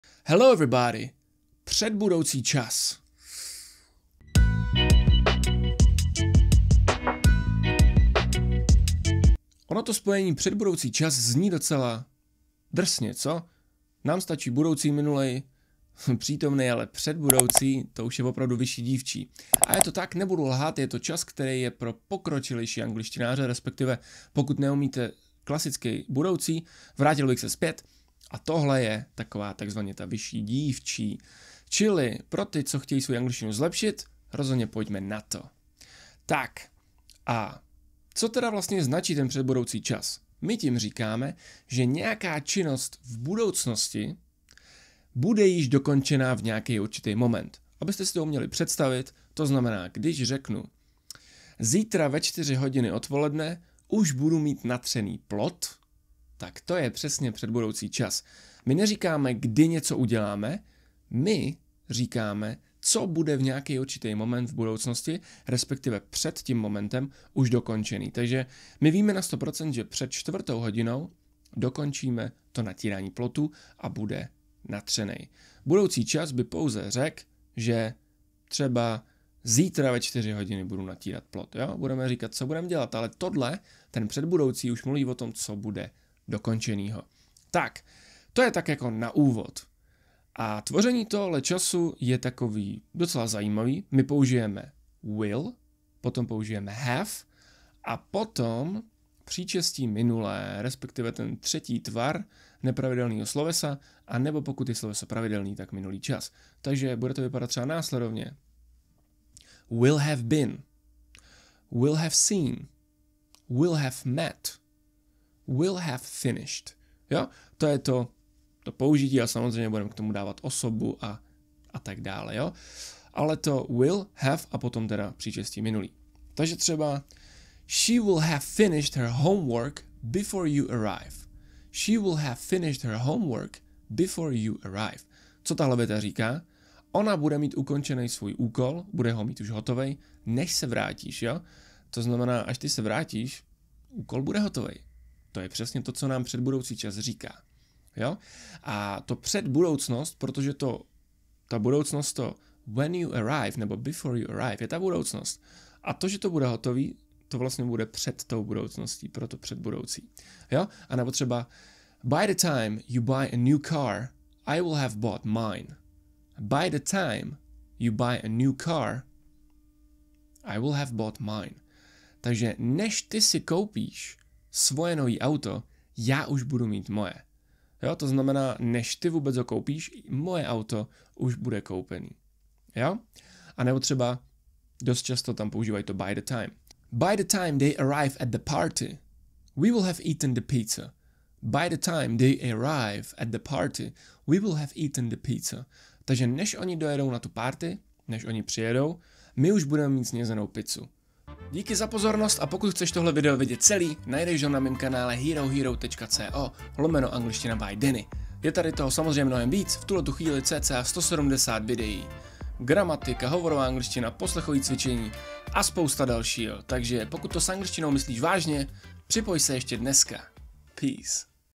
Přestaň se bát PŘEDBUDOUCÍHO ČASU a nauč se ho používat!
Dáme si jednu pokročilejší lekci na téma "Předbudoucí čas". Je to sice celkem pokročilá anglická gramatika, ale ty to zvládneš. 💪

Pochop základy této problematiky a buď mezi angličanama za borce! 🇺🇸🇬🇧